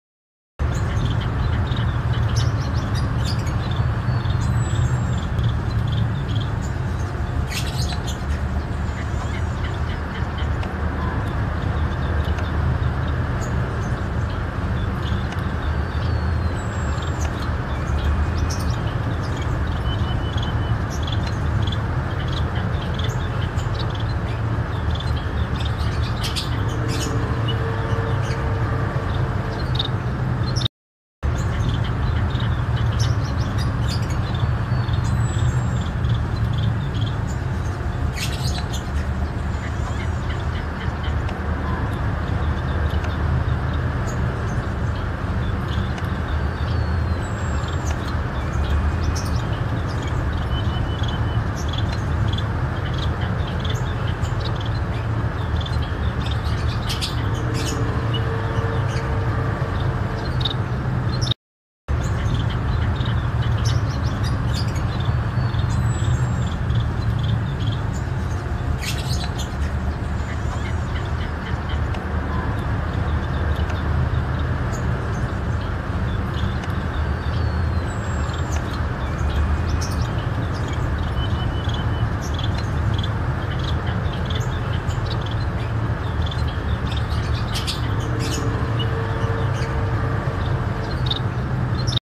Bondol haji ngriwik
Burung  hama yg di pandang sebelah mata
Kini banyak di cari dan menjadi primadona
Emprit Bondol kaji (haji) yg dalam bahasa latinnya lonchura maka
Burung pemakan biji-bijian ini hidup secara berkelompok dan msh sering kita jumpai di ladang/persawahan.
Burung emprit Bondol haji ini bisa di masterin/bisa menirukan suara burung lain
Misal suara kenari
Suara perkutut
Suara prenjak dll